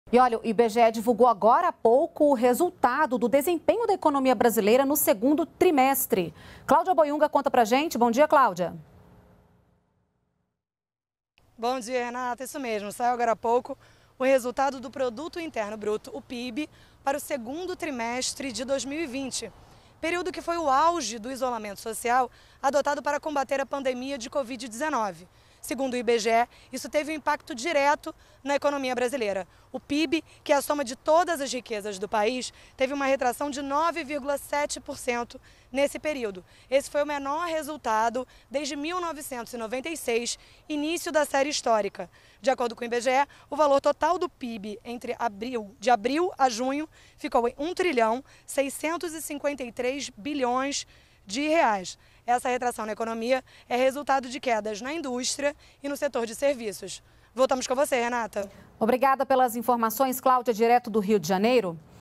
PIB recua 9,7% no 2º trimestre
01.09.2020 - O IBGE divulgou, agora há pouco, o resultado do desempenho da economia brasileira no segundo trimestre do ano.